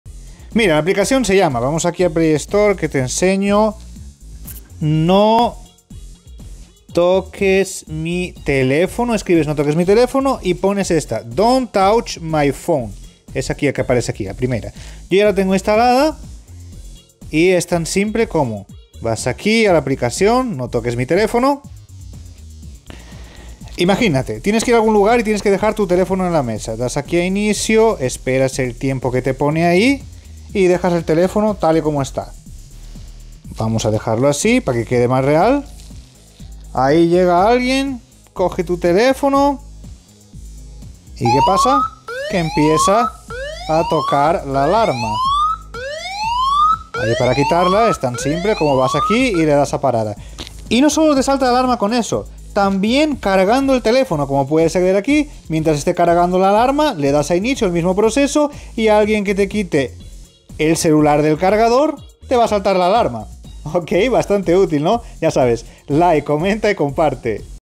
CON ESTE TRUCO SABRÁS QUIEN TE ANDUVO EN EL TELÉFONO
Con esta alarma sabrás quién cogió tu teléfono sin tu permiso 😉😃🤙